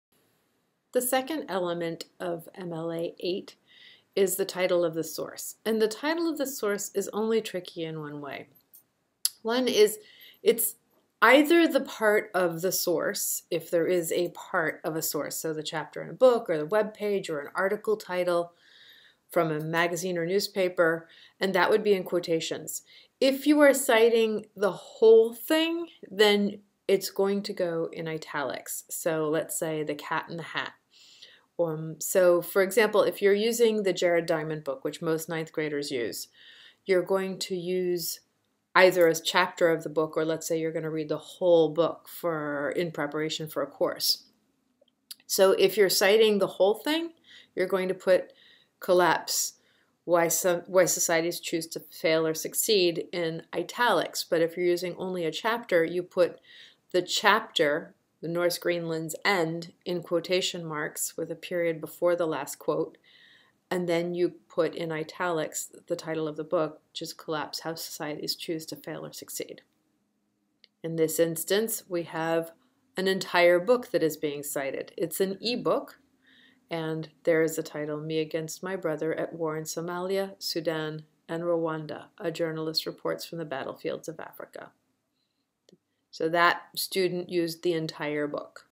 MLA 9 Self Guided Lesson (Element 2 - Title)
Number 3 of 14
This video provides MLA 8 guidelines, which apply to MLA 9 as well. This is one of 14 tutorials meant to help students learn how to cite sources in MLA 9. It was designed as part of a student-paced instructional experience for high school students. It can be administered via Nearpod or Moodle with embedded checks (in the form of quiz-like questions) after each video. Teachers administer a five minute quiz after all students have worked through the entire series (either at home or in class).